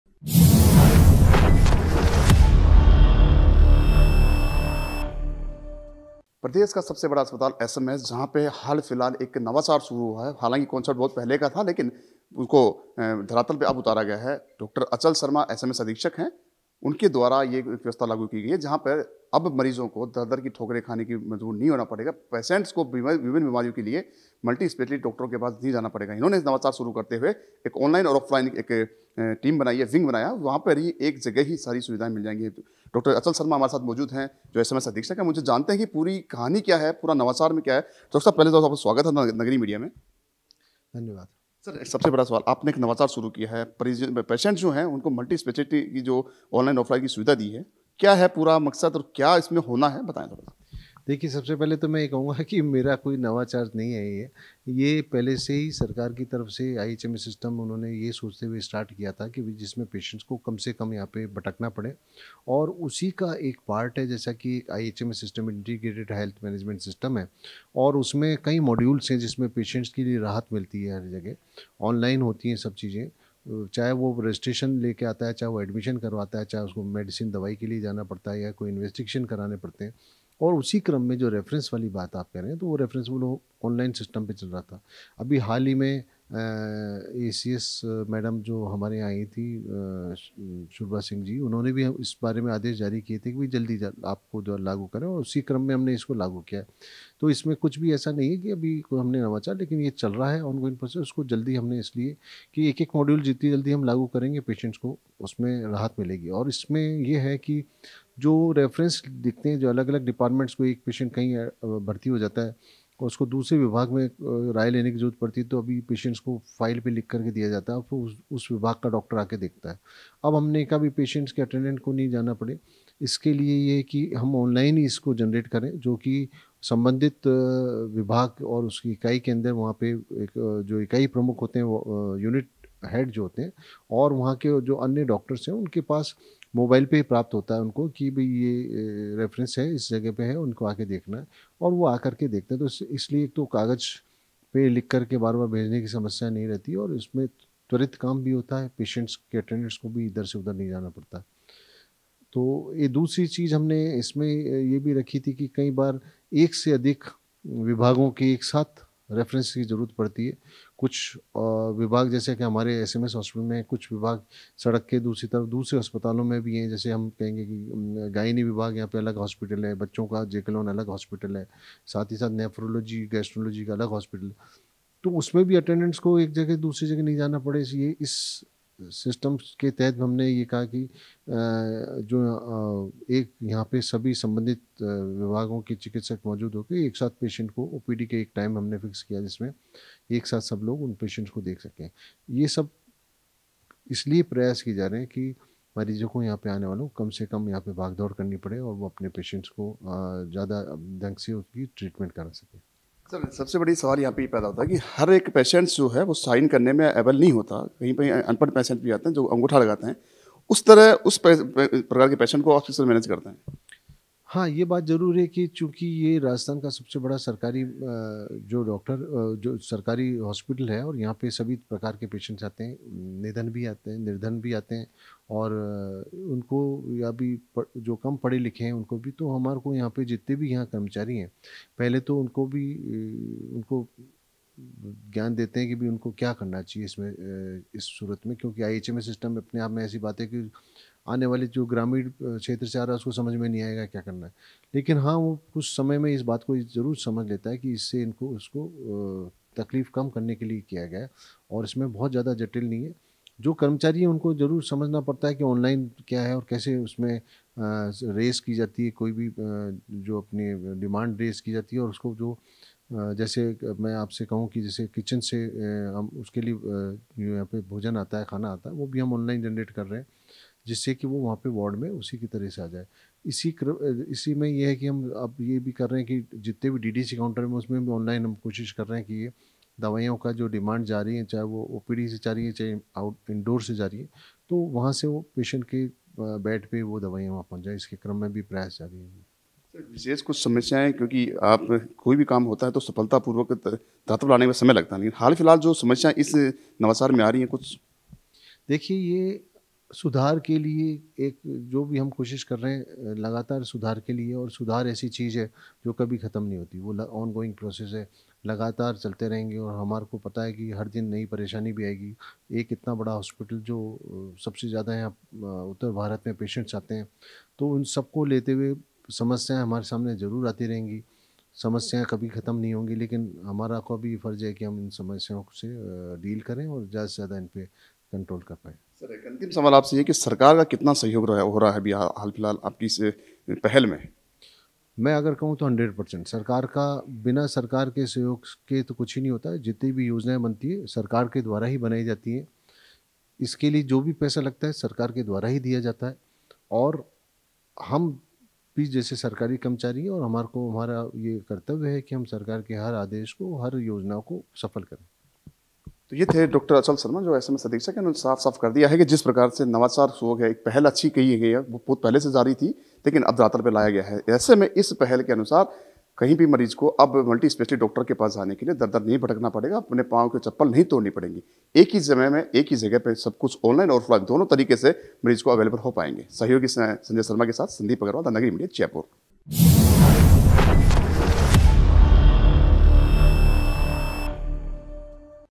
प्रदेश के सबसे बड़े अस्पताल SMS में अब एक ही जगह मिलेंगे मल्टी स्पेशलिस्ट....
About this video :
प्रदेश के सबसे बड़े अस्पताल स्वाइन मान सिंह में अब एक ही जगह मिलेंगे मल्टी स्पेशलिस्ट.